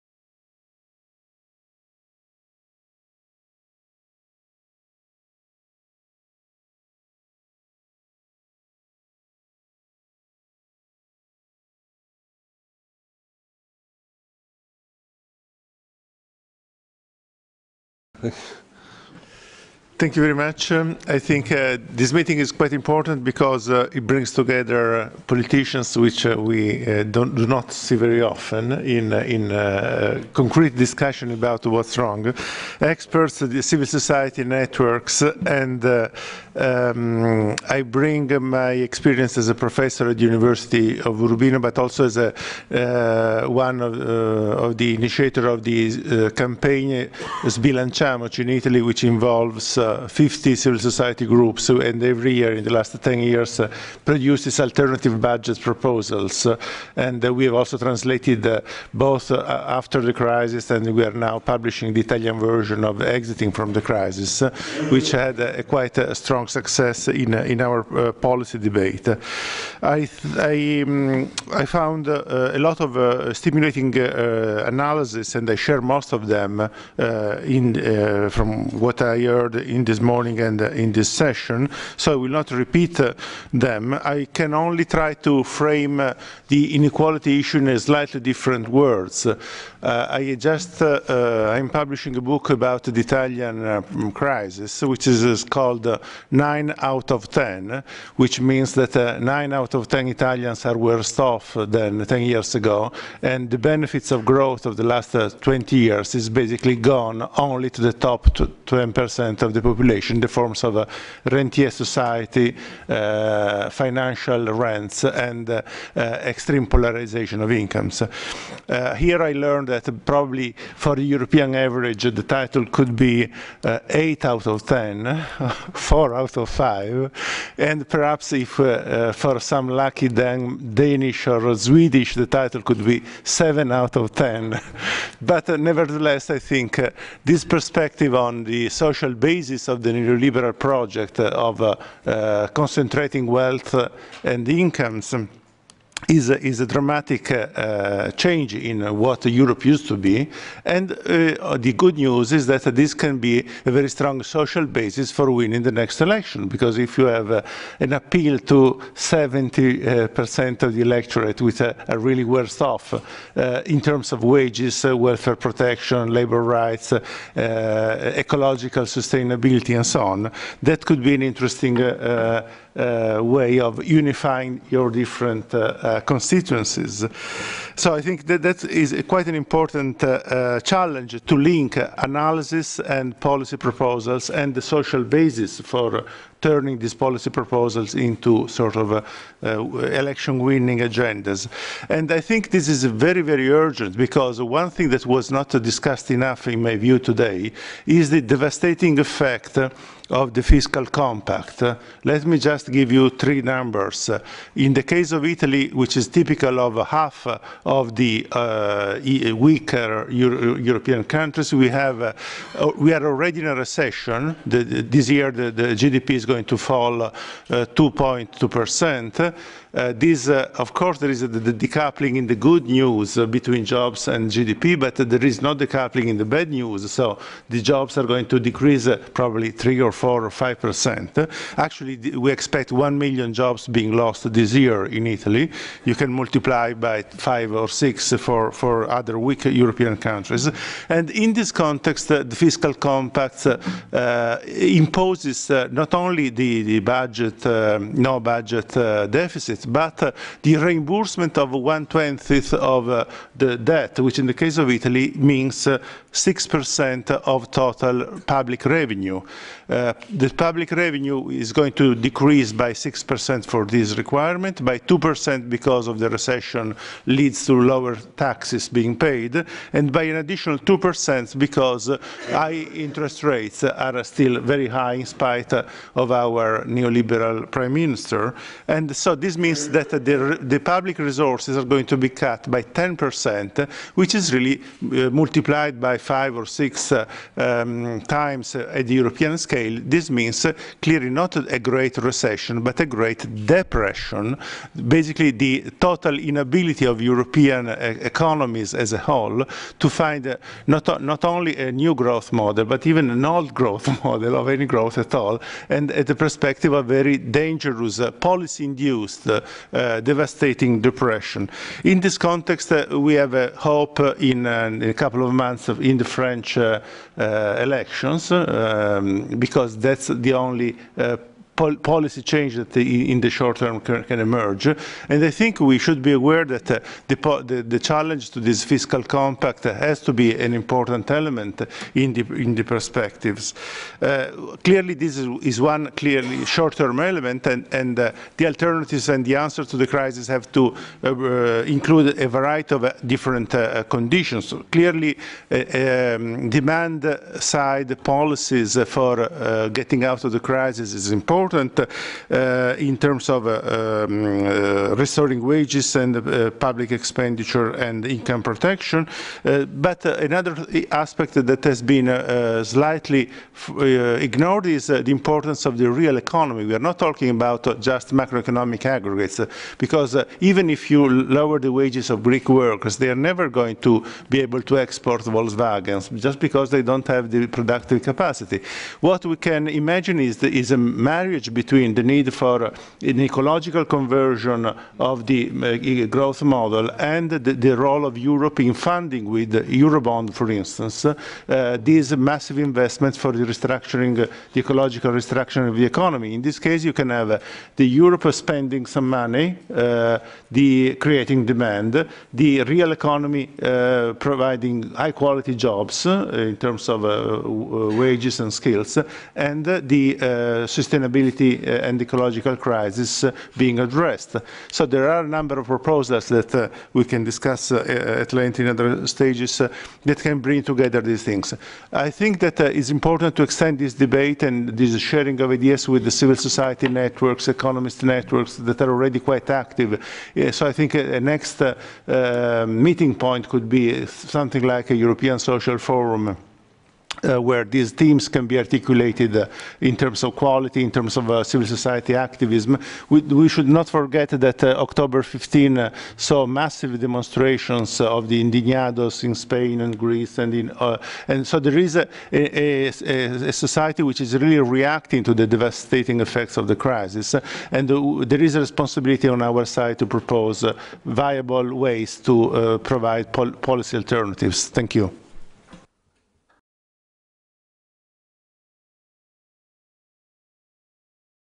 26 - New Growth Model (Mario Pianta, Professor University of Urbino)
Conference co-organised by the ETUI and the European Parliament on 9 February 2012